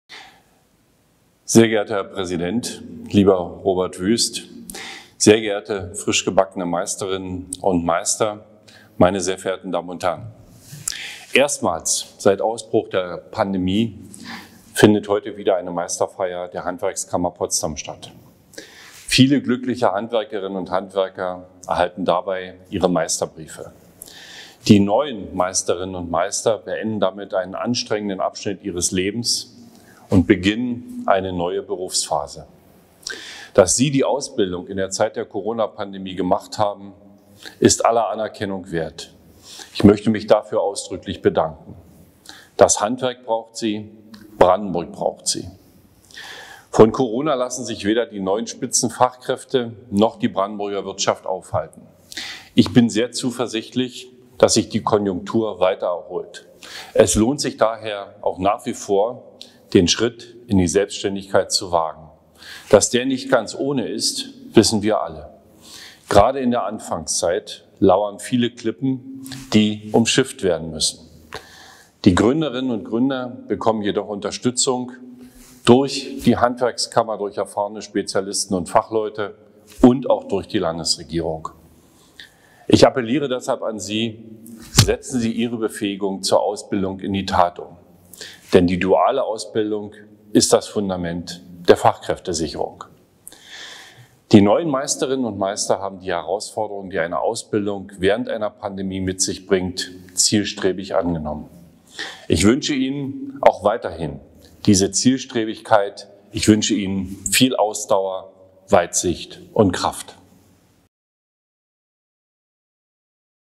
Grußwort des Ministerpräsidenten der Landes Brandenburg zur Meisterfeier der Handwerkskammer Potsdam
Ministerpräsident Dr. Dietmar Woidke gratulierte am 28. August 2021 den Meisterinnen und Meistern: „Die wirtschaftliche Entwicklung in unserem Land ist meisterlich! Jungmeisterinnen und Jungmeister wie Sie tragen einen besonderen Anteil an dieser Entwicklung. Dafür danke ich Ihnen heute ganz besonders. Sie haben trotz der pandemischen Herausforderungen gezeigt, dass ein robustes Handwerk das Rückgrat der brandenburgischen Wirtschaft bildet. Ebenso ist die Ausbildungssituation in Brandenburg dank der vielen engagierten Ausbildungsbetriebe und modernen Ausbildungsstätten aussichtsvoll und macht Mut, denn das Land braucht dringend Fachkräfte. Immer mehr junge Menschen erkennen: Gute Arbeit gibt es hier in der Heimat Brandenburg. Sie als Fachkräfte sind die Triebfeder für Stabilität und eine gute Entwicklung in unserem Land. Danke für Ihr Engagement. Das Land Brandenburg setzt auf Sie!“